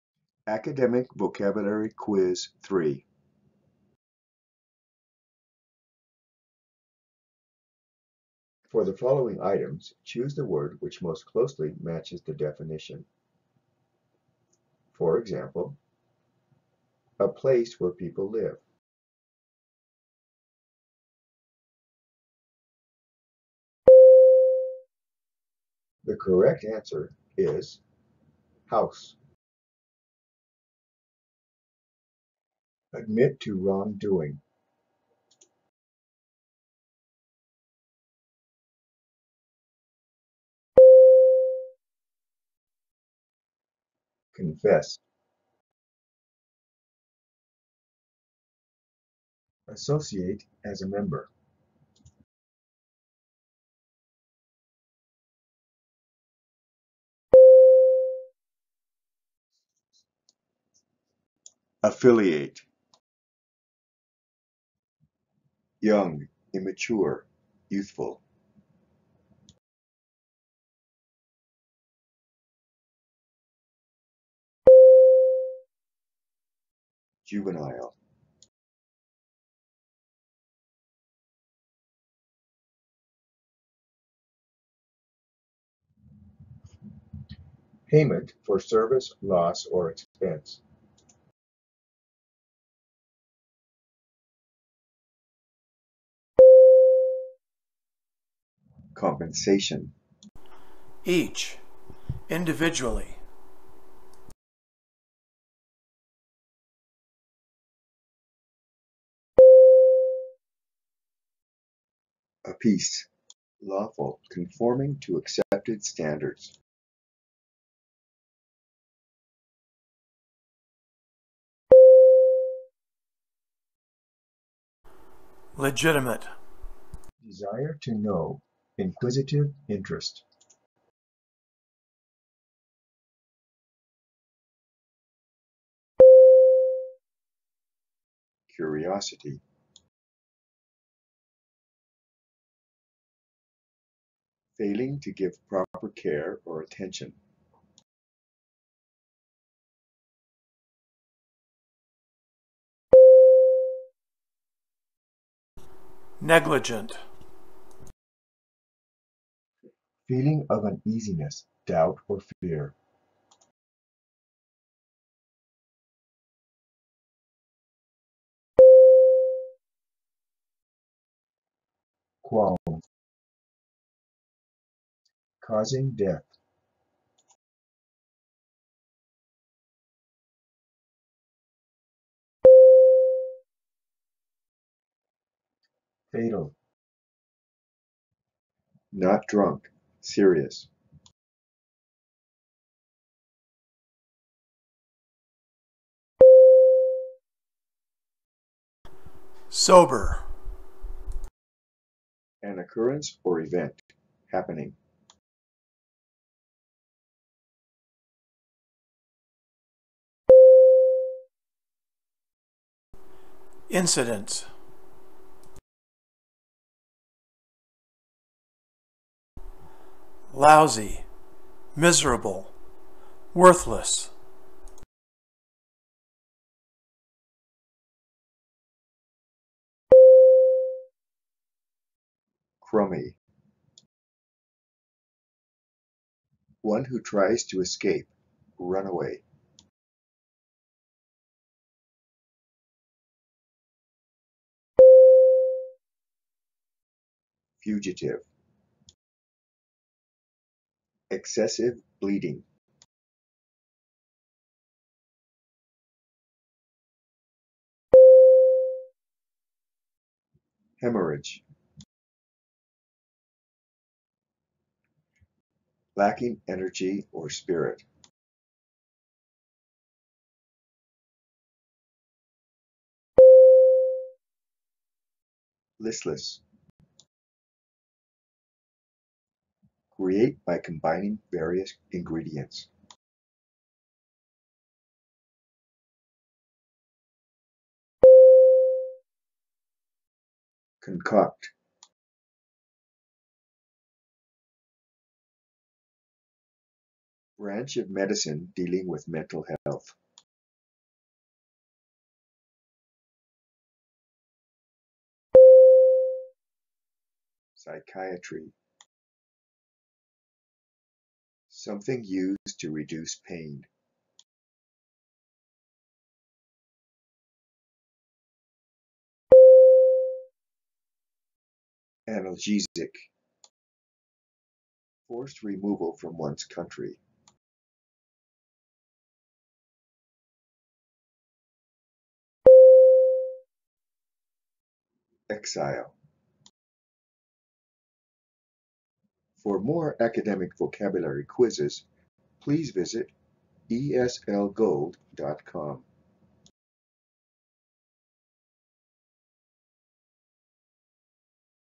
Test Your English: Academic Vocabulary Quiz #3
Now you can learn English quickly by yourself at home for free step by step!  This video gives practice with common academic vocabulary words in English.  Excellent for teachers and students of English as a Second Language (ESL).  Check out your vocabulary knowledge and skill with these academic tests.